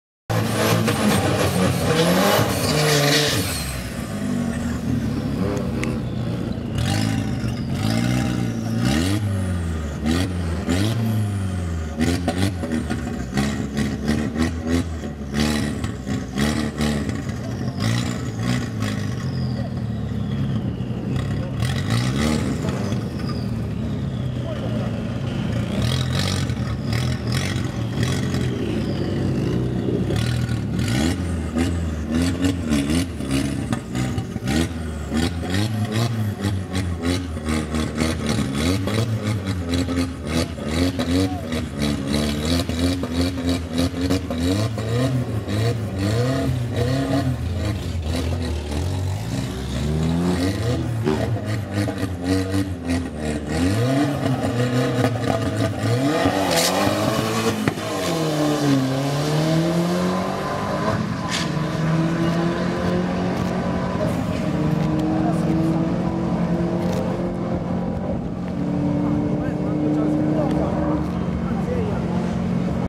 June 2018 QTM Day 3 Run 04
03rd June 2018 - Day 3, Eliminations and Finals for the 2nd Quarter Mile event in the MDRA calendar for this year.